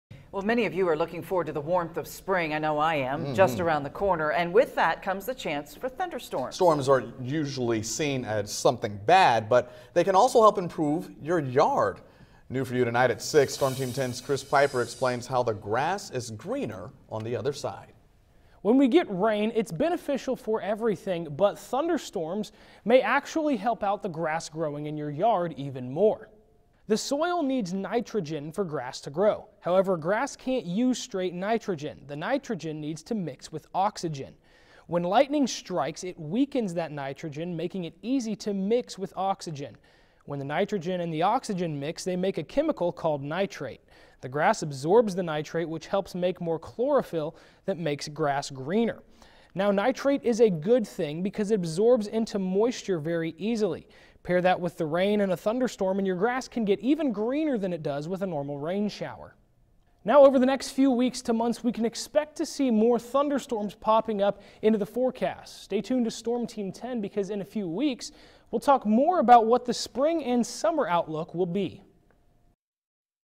Lightning Makes Grass Greener
A normal rain shower can make grass green, but the lightning in a thunderstorm can make it even greener.